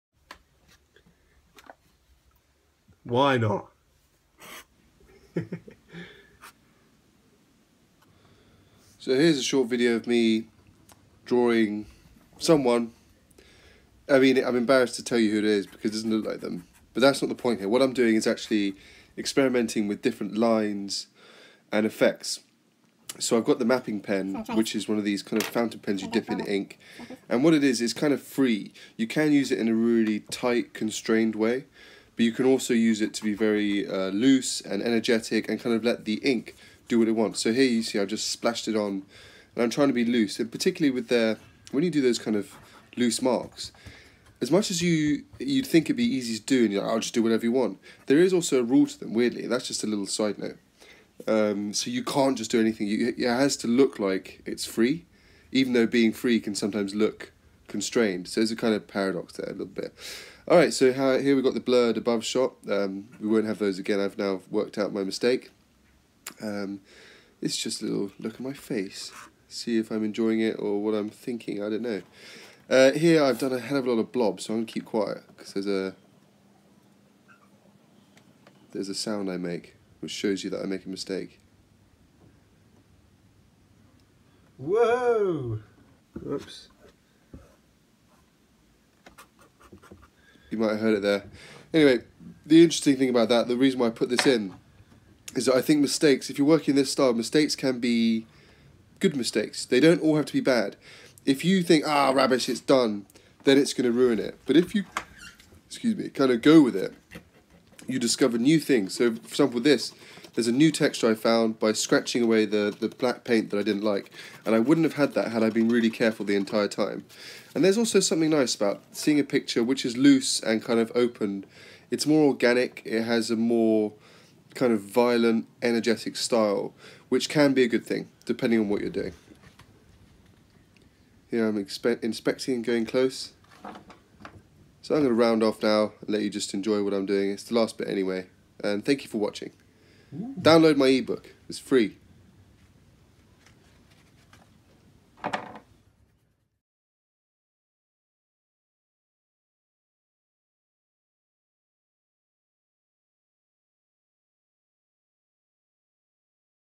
Sam Smith drawing Conor McGregor (badly)
A video of me experimenting with pen and ink doing a questionable portrait of Conor McGregor.